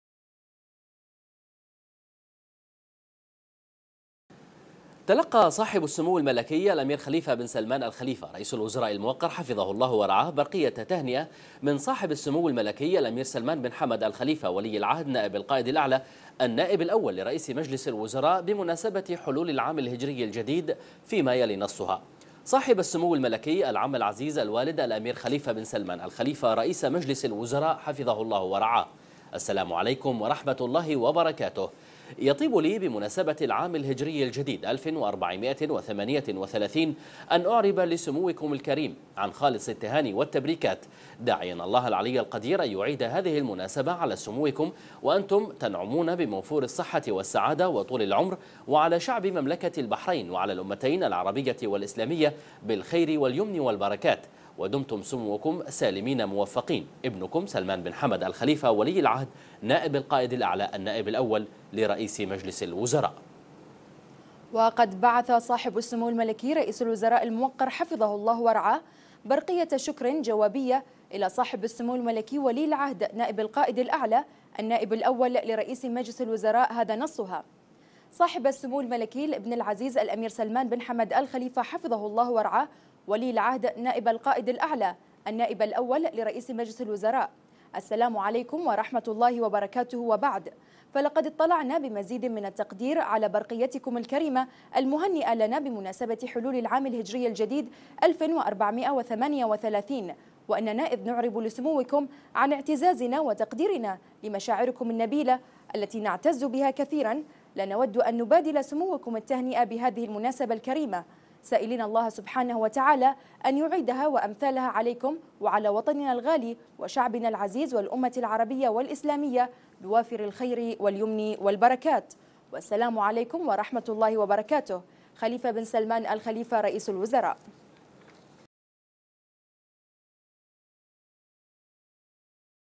البحرين : سمو رئيس الوزراء يتلقى برقية تهنئة من سمو ولي العهد
الوصف

سمو رئيس الوزراء يتلقى برقية تهنئة من سمو ولي العهد